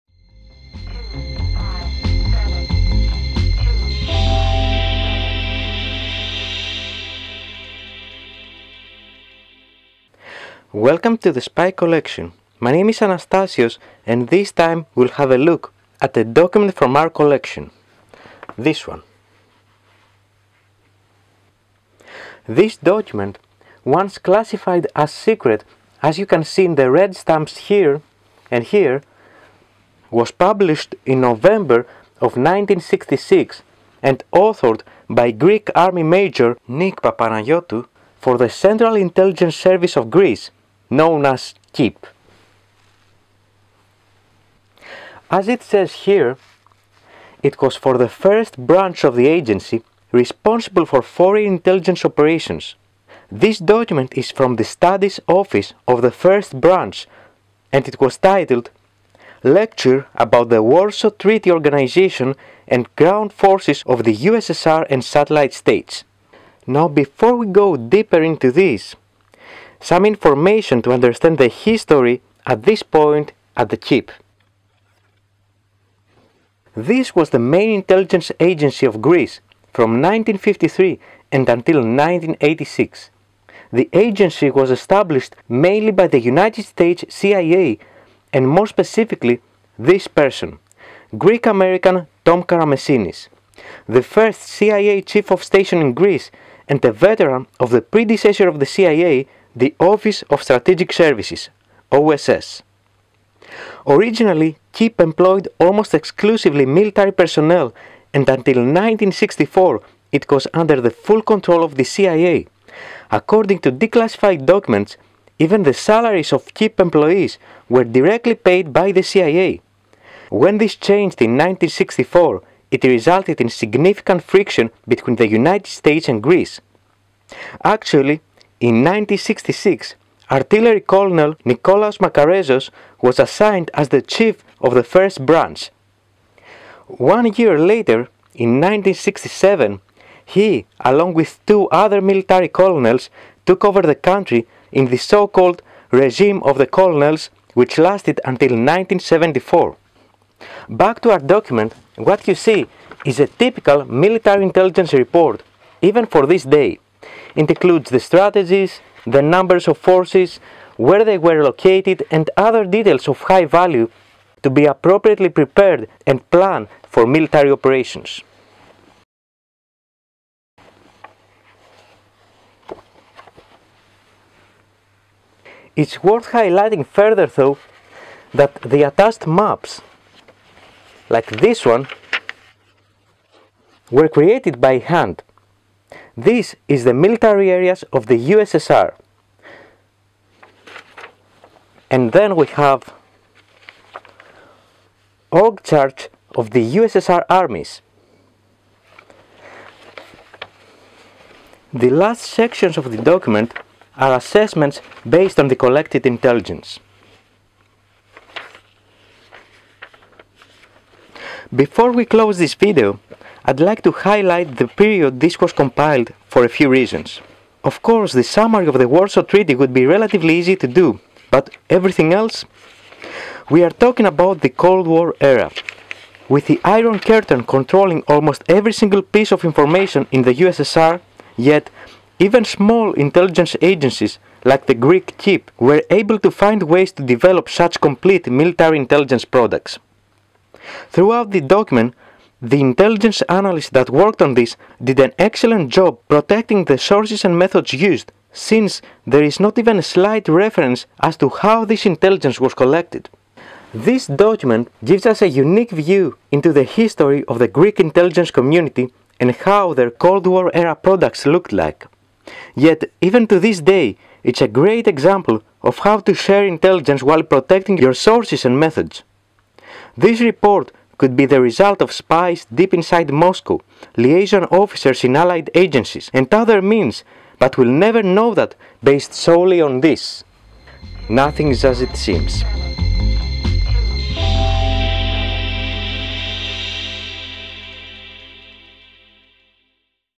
Greek KYP (ΚΥΠ) 1966 Study on the Warsaw Treaty Organisation
This is a rare intelligence report from the Central Intelligence Service (KYP - ΚΥΠ) of Greece from 1966, presenting the Warsaw Treaty Organisation and the ground forces of the USSR and satellite states. The document was produced during a key time in the history of KYP, right after its separation from the control of the United States CIA, and right before the Head of the First Branch participated in the military dictatorship that ruled Greece from 1967 to 1974.

00:00 - Opening
00:10 - Document overview
01:17 - History
02:30 - Document content
03:31 - Conclusion
04:40 - Closing